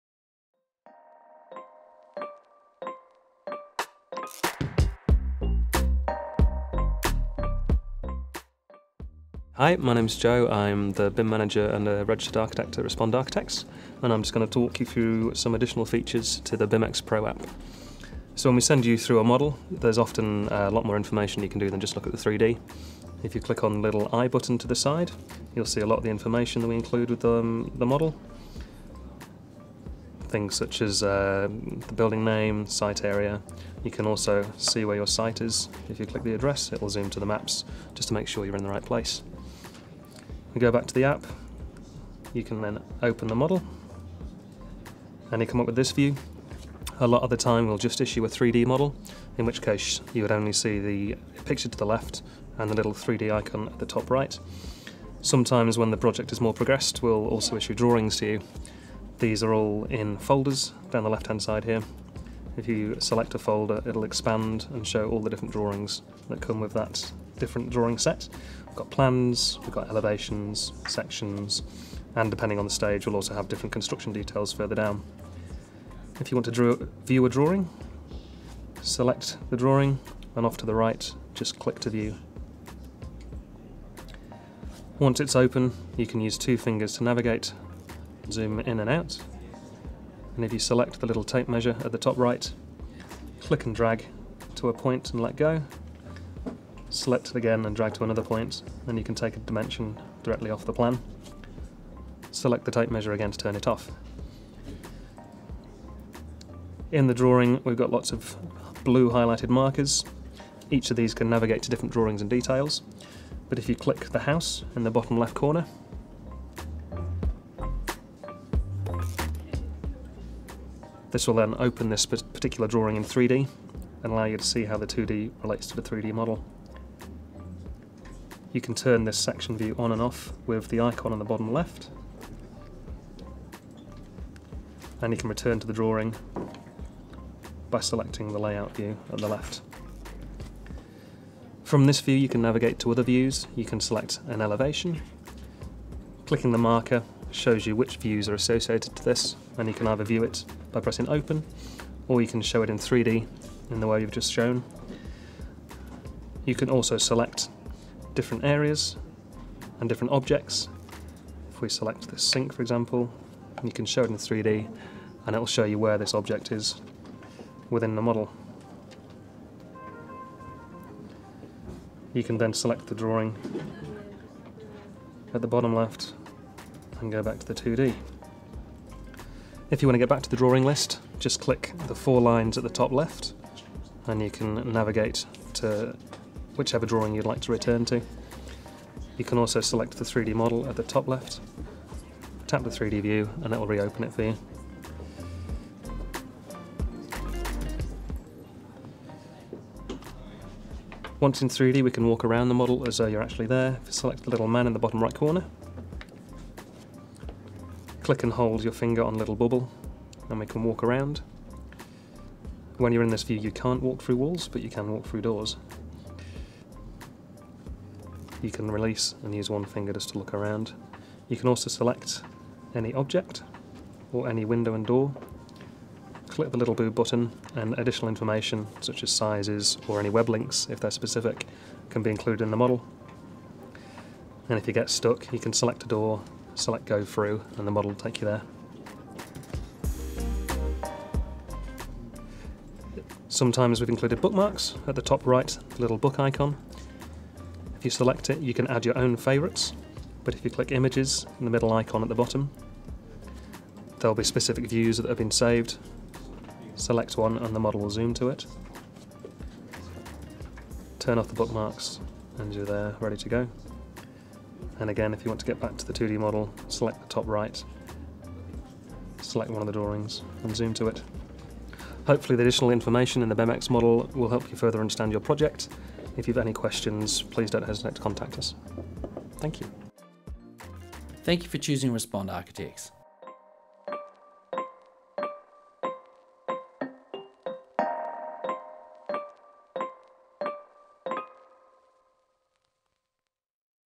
How to Use BIMx on an iPad - 2019
Registered Architect and BIMx Manager from Respond Architects in Auckland demonstrates how to use the BIMx app on an ipad to view plans and walk through your projects in 3D.

The 3D walkthrough is a great way to visualise 2D plans for clients. Individual items in the plans can be tagged with any information you need including dimensions, locations and quantities.

If you'd like to see your own plans brought to life, speak to Respond Architects today